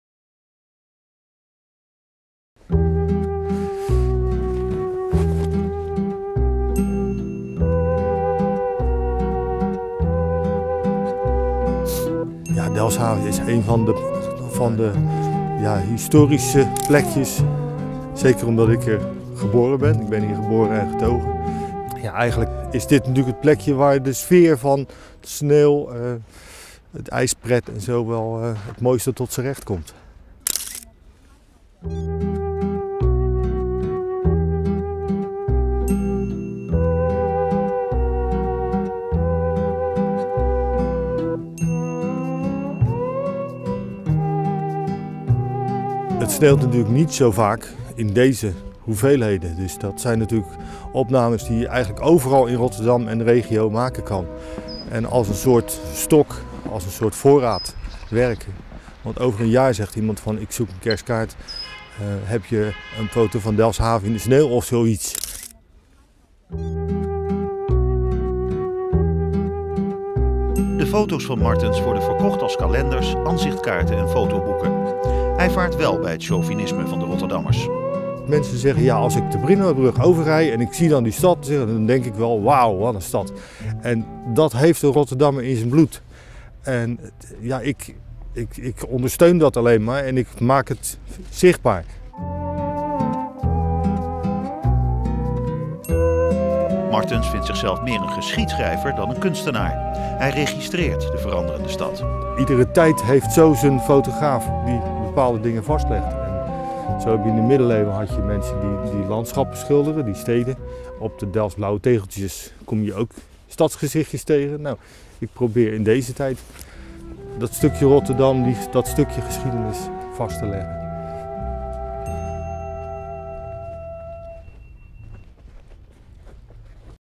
Fotograaf Paul Martens haalt inspiratie uit winterweer
Als de stad wit is, houdt niets de Rotterdamse fotograaf Paul Martens nog tegen. 'Camera mee en er op uit'. En het zal u niet verbazen dat zijn foto's vooral kalenders en kerstkaarten zullen sieren! oor vandaag maakte hij een logische keuze: Delfshaven.